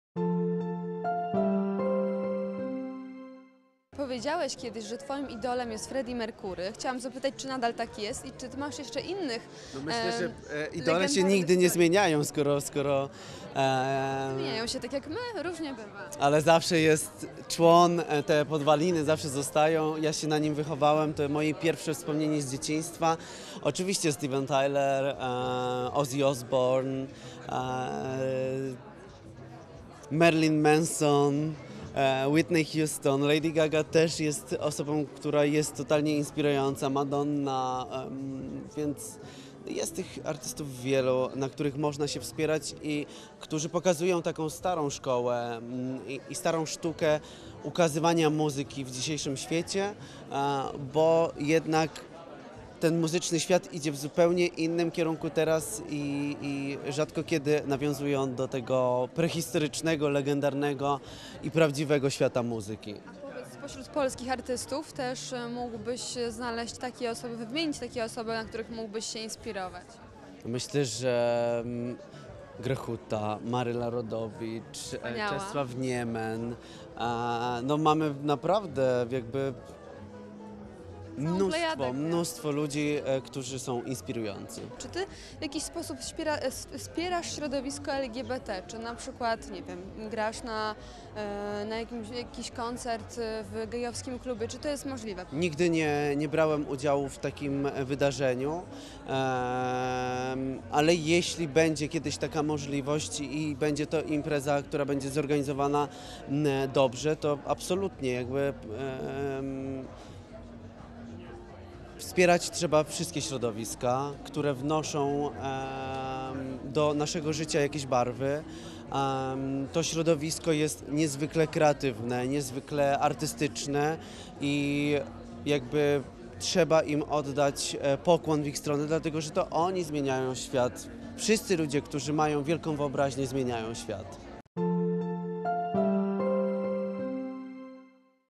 Michał Szpak ciepło o środowisku LGBT: Trzeba oddać im pokłon, bo zmieniają świat | Wideoportal
Michał Szpak przypomniał, że jego wielkim idolem jest Freddie Mercury. Zadeklarował też, że wspiera gejów i lesbijki. Chętnie wystąpiłby w gejowskim klubie.

TOP 10